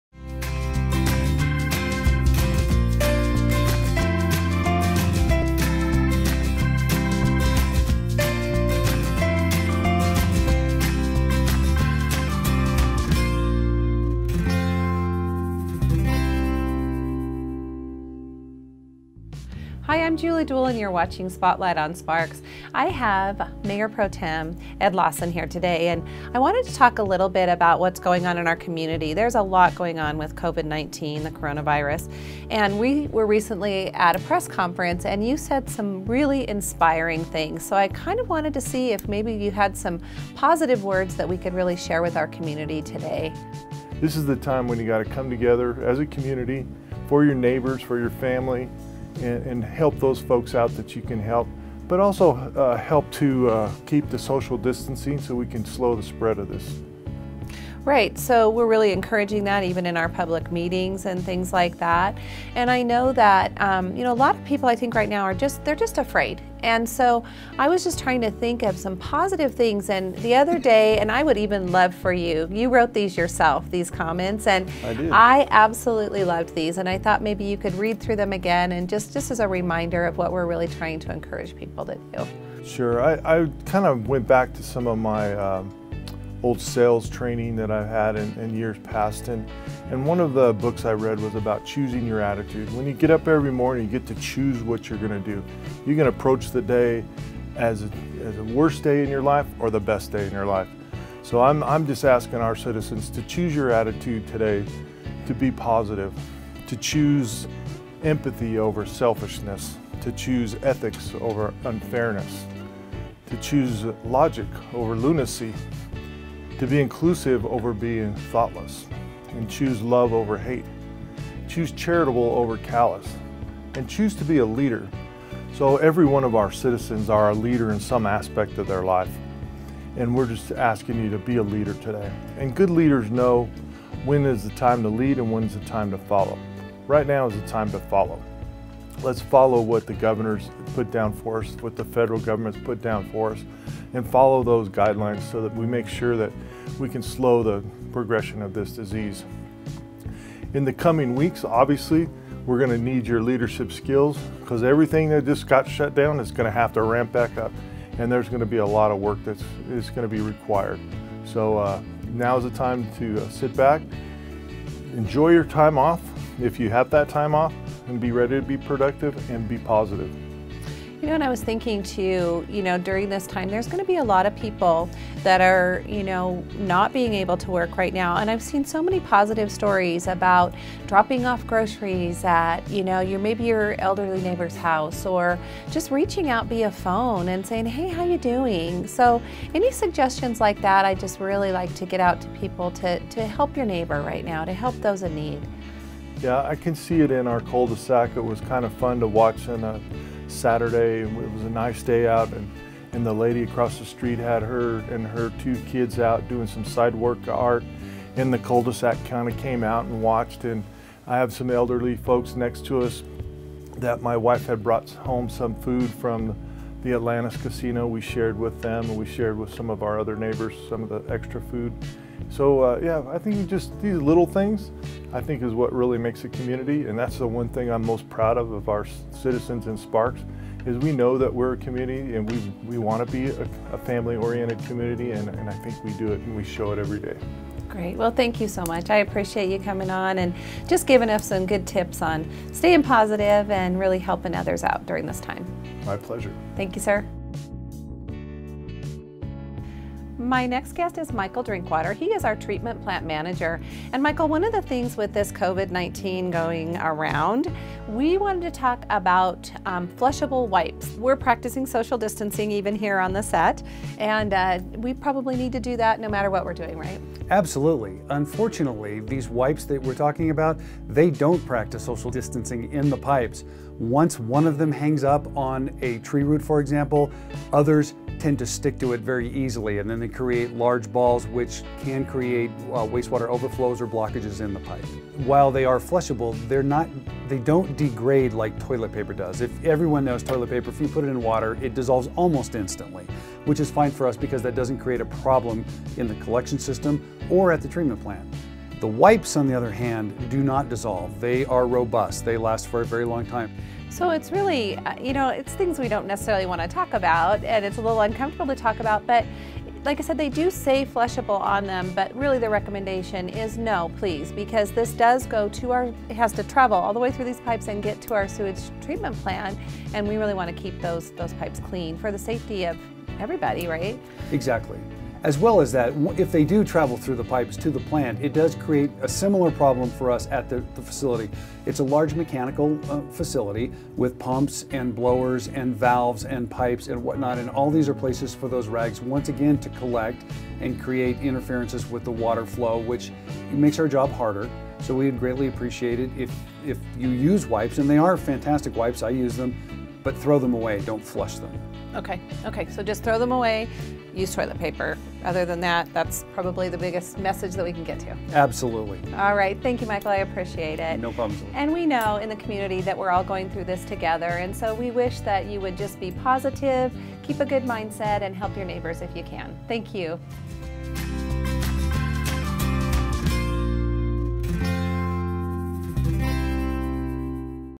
Spotlight March 2020
Our Sparks City Councilman Ed Lawson speaks to being positive during this COVID-19 pandemic. And our treatment plant manger Michael Drinkwater talks about not flushing "flushable" wipes down the toilet.